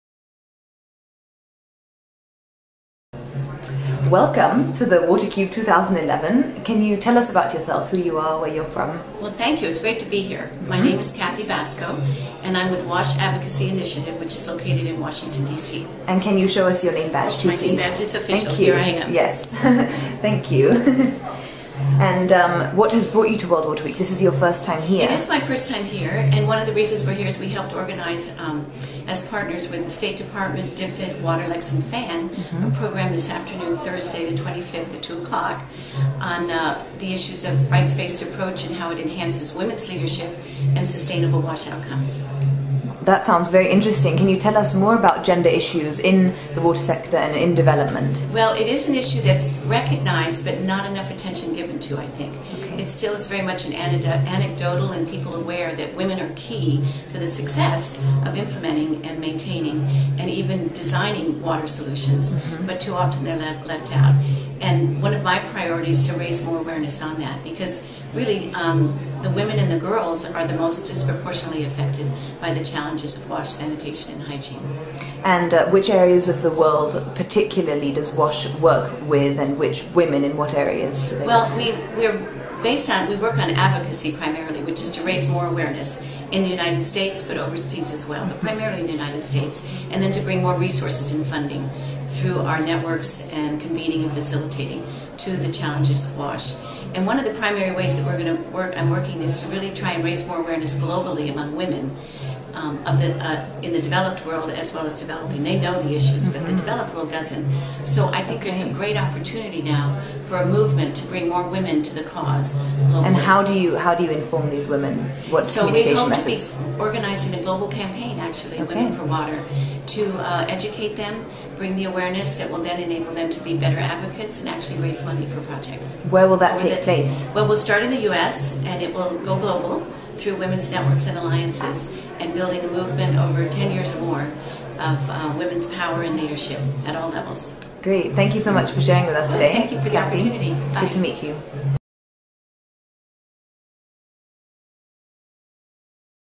Cathy Baczko of Women for Water
Cathy Baczko, who joins us at Stockholm World Water Week for the first time from Washington D.C., reminds us of the importance of recognising women's role in water development and their potential in creating water solutions. She discusses how vital it is to raise awareness about water sanitation issues especially for women in developed countries which is why the WASH Advocacy Initiative is organising the global campaign called Women for Water. Watercube interview by Helen Downey at Stockholm World Water Week on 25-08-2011. (Aug 25, 2011)